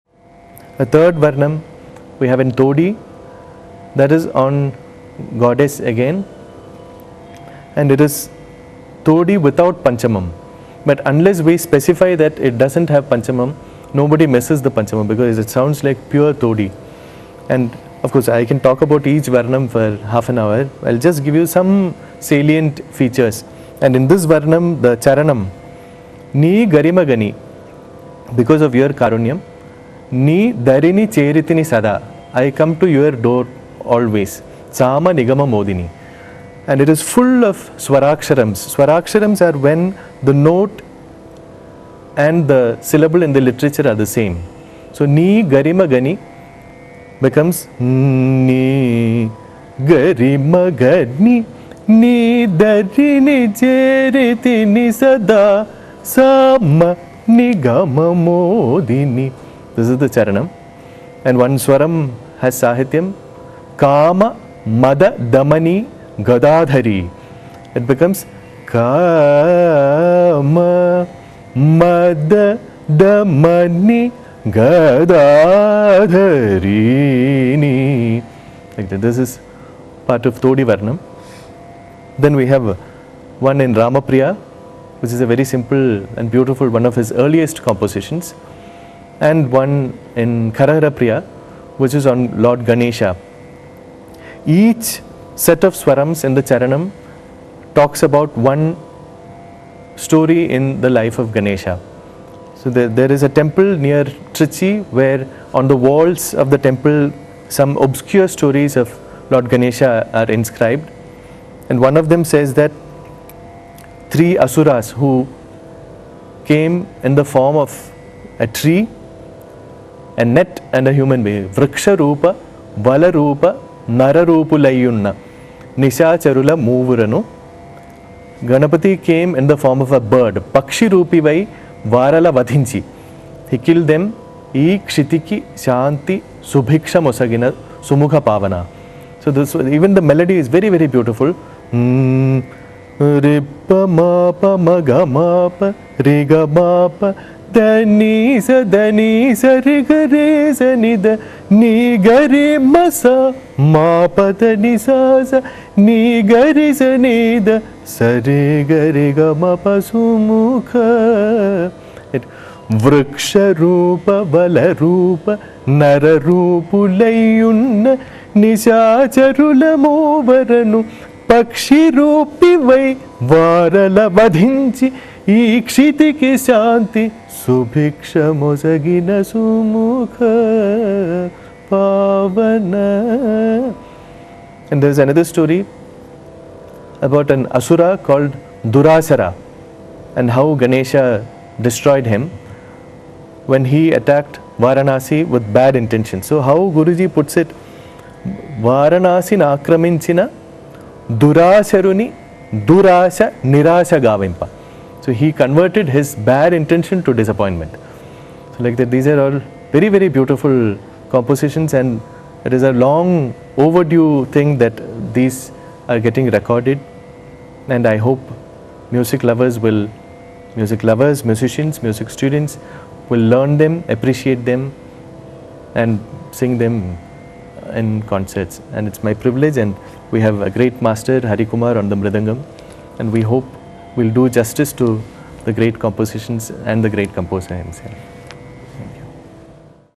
Prince Rama Varma talks about Dr.M.Balamuralikrishna's Varnams 2/2
Prince Rama Varma is probably the only person who has given both vocal and veena support to his guru Dr.Balamuralikrishna's singing and has also sung, with Dr.Balamuralikrishna accompanying him on the viola! Here is a track from the recently released DVD of the five Pada Varnams of Dr.Balamuralikrishna, where Varmaji offers some insights into his guru's Varnams. I have heard Varmaji throw light on the intricacies on the compositions of Dr.Balamuralikrishna in a way few others can. This DVD should be available for sale at places like the Karnatic Music Book Centre, Sripuram 1st Street, Chennai, sooner than later.